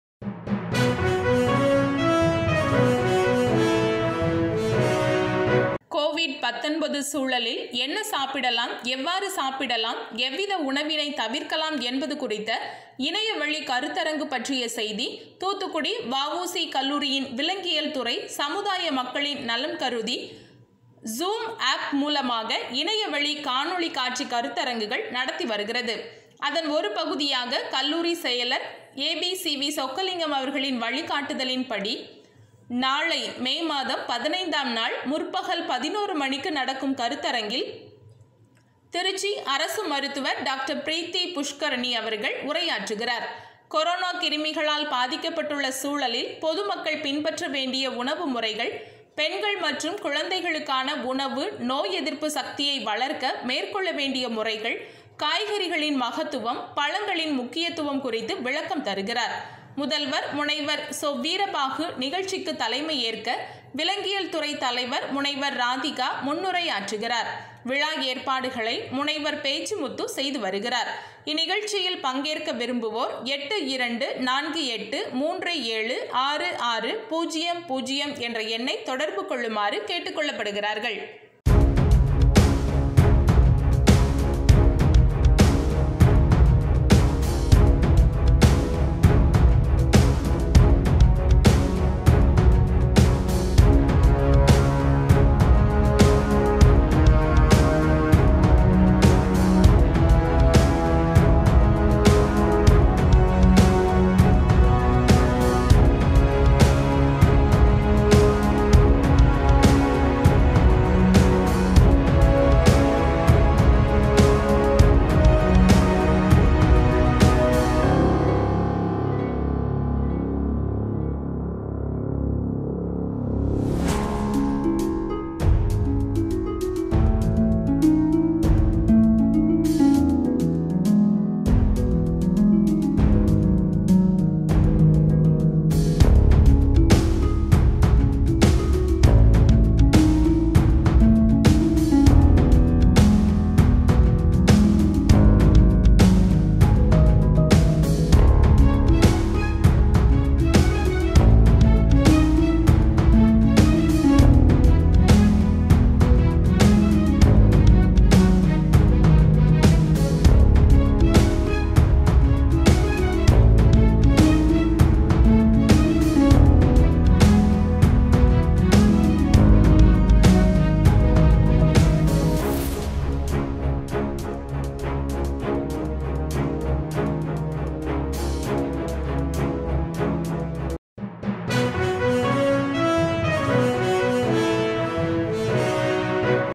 கோவிட் 19 சூழலில் என்ன சாப்பிடலாம் எவ்வாறு சாப்பிடலாம் எவ்வித உணவை தவிர்க்கலாம் பற்றிய கருத்தரங்கு
VOCCOLLEGE TUTICORIN ZOOLOGY DEPARTMENT ZOOMMEETING CORONAVIRUS FOOD CONFERENCE கோவிட் 19 சூழலில் என்ன சாப்பிடலாம்?? எவ்வாறு சாப்பிடலாம்? எவ்வித உணவினைத் தவிர்க்கலாம் என்பது
குறித்த இணைய வழி கருத்தரங்கு பற்றிய செய்தி!!
தூத்துக்குடி வஉசி கல்லூரியின் விலங்கியல்துறை, சமுதாய மக்களின் நலம் கருதி ZOOM  app மூலமாக இணையவழி காணொளிக்
காட்சி  கருத்தரங்குகள் நடத்திவருகிறது.அதன் ஒரு பகுதியாக,
கல்லூரி செயலர் ஏபிசிவீ சொக்கலிங்கம் அவர்களின் வழிகாட்டுதலின்
படி ,நாளை மேமாதம் 15ஆம் நாள் முற்பகல் 11 மணிக்கு நடக்கும் கருத்தரங்கில் திருச்சி அரசு மருத்துவர் டாக்டர்.பிரீத்தி புஷ்கரிணி அவர்கள் உரையாற்றுகிறார்கொரானா கிருமிகளால் பாதிக்கப்பட்டுள்ள சூழலில் பொதுமக்கள் பின்பற்ற வேண்டிய உணவு முறைகள்,
பெண்கள் மற்றும் குழந்தைகளுக்கான உணவு,நோய் எதிர்ப்பு சக்தியை வளர்க்க மேற்கொள்ள வேண்டிய முறைகள்,காய்கறிகளின் மகத்துவம், பழங்களின் முக்கியத்துவம் குறித்து விளக்கம் தருகிறார்.
முதல்வர் முனைவர் சொ.வீரபாகு நிகழ்ச்சிக்கு தலைமைஏற்க,
விலங்கியல்துறை தலைவர் முனைவர் ராதிகா முன்னுரையாற்றுகிறார்.விழா ஏற்பாடுகளை முனைவர் தெ,பேச்சிமுத்து செய்து வருகிறார்.
இந்நிகழ்ச்சியில் பங்கேற்க விரும்புவோர் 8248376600 என்ற எண்ணை தொடர்பு கொள்ளுமாறு கேட்டுக்கொள்ளப்படுகிறார்கள்!!